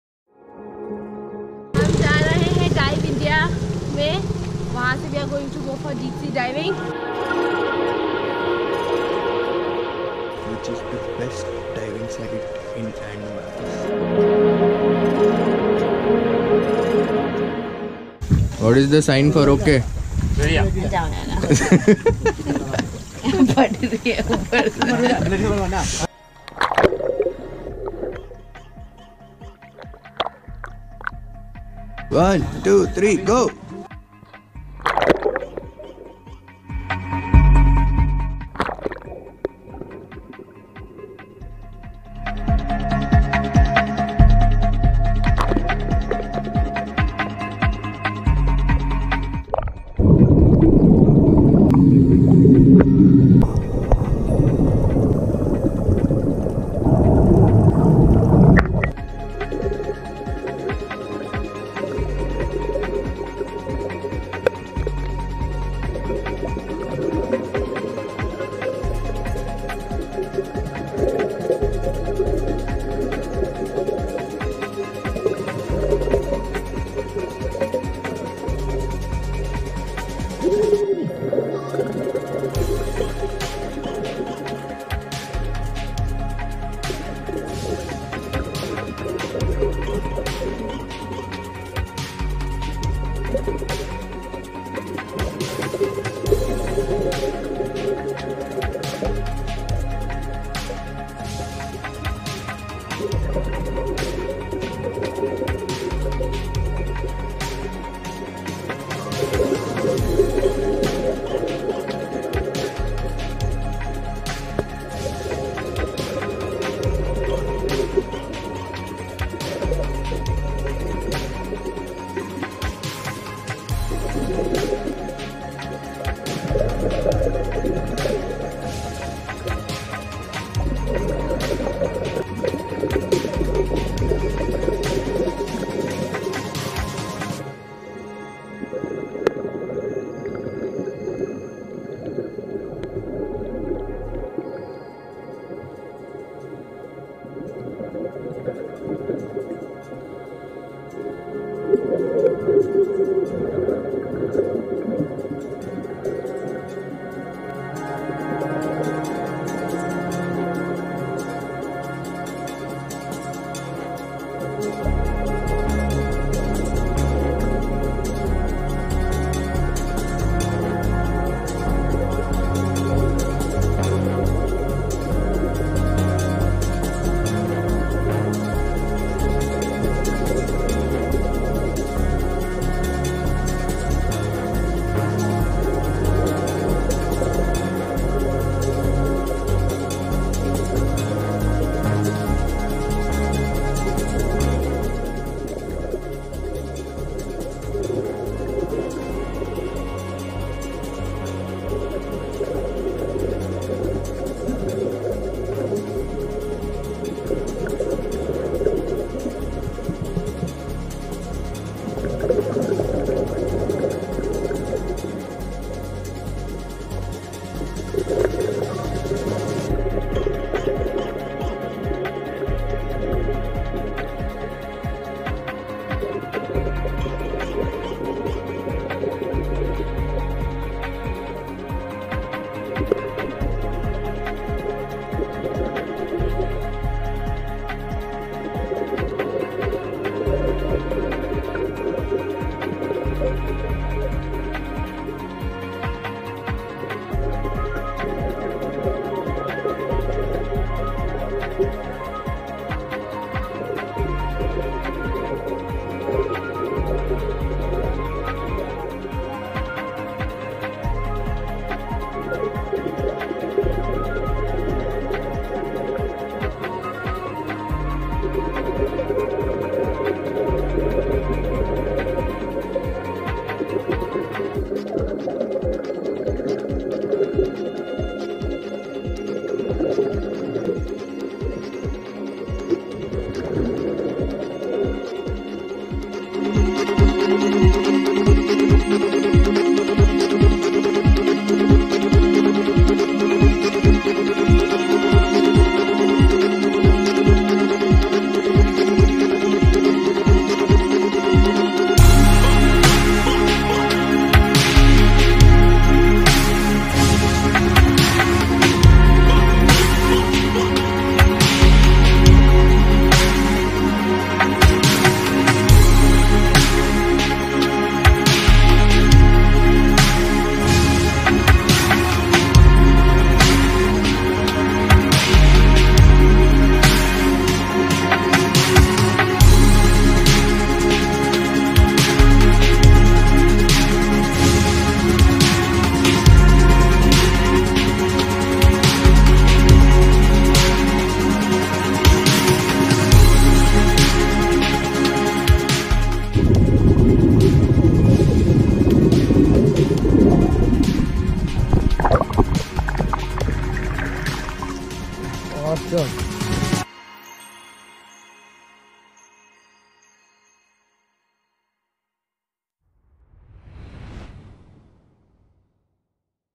Boat dive with DiveIndia @ Havelock (Andaman Islands - Part 5)
Scuba Dive with Dive India center at Havelock island